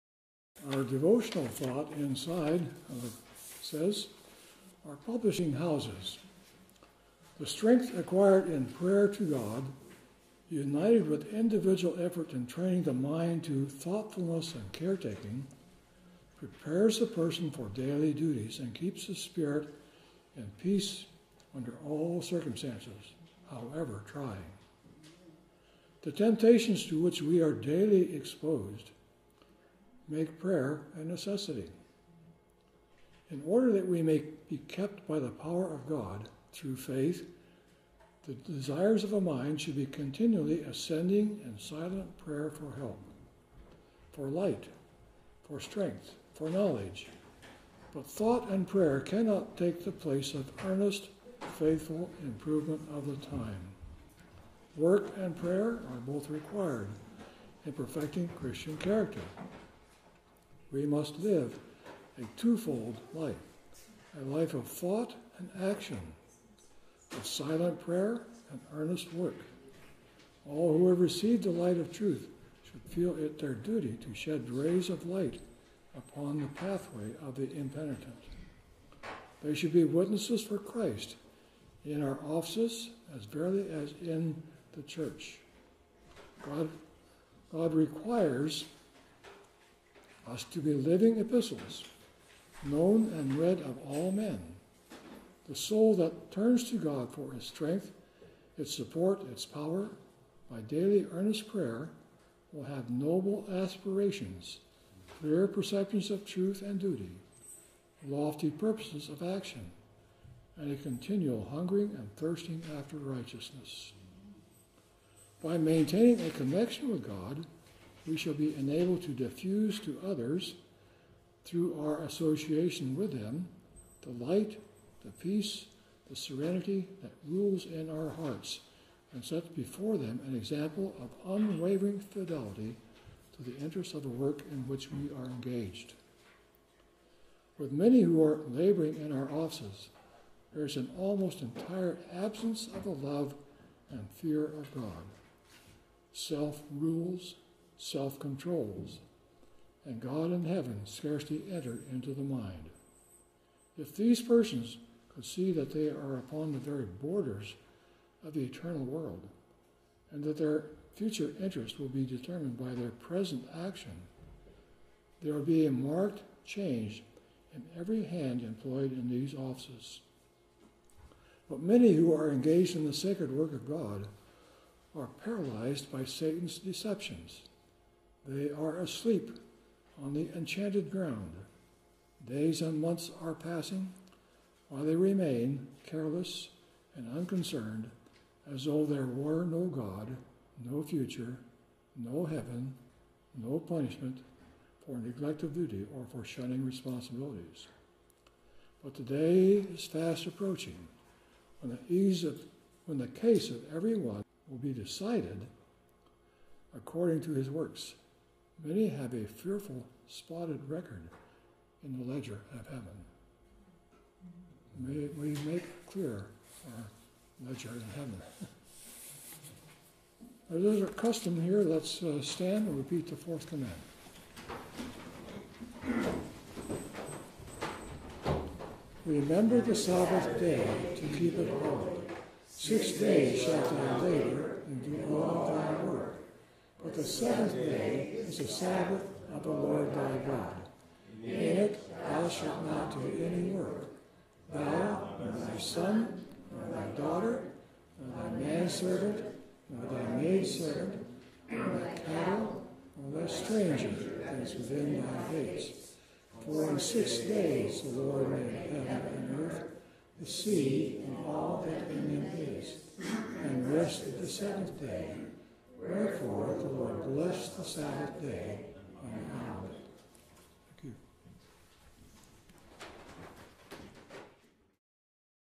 Our Publishing Houses, T4 Spirit of Prophecy -Roger Nelson
Recorded at The Village Lighthouse Church in Croton, OH.

Be sure to also visit our church Look for our speaker schedule and for more information about our ministry.